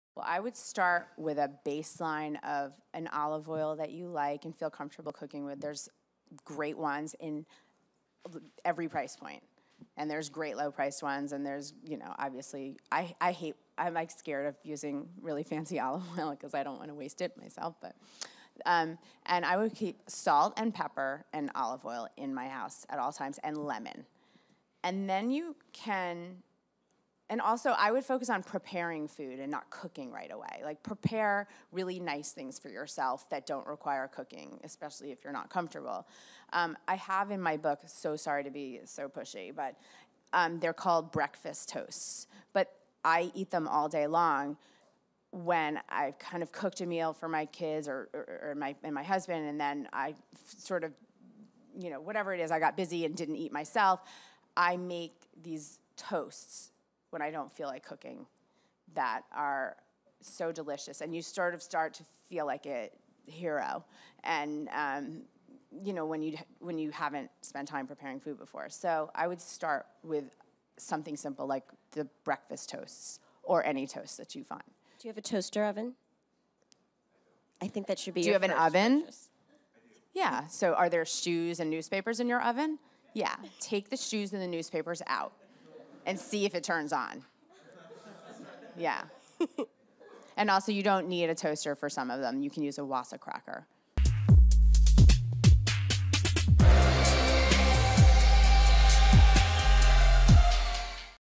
Jessica Seinfeld On The Essential Ingredients You Should Have
Jessica Seinfeld discusses the must have ingredients every cook should have in their kitchen.

Moderated by Sierra Tishgart (New York Magazine/Grub Street)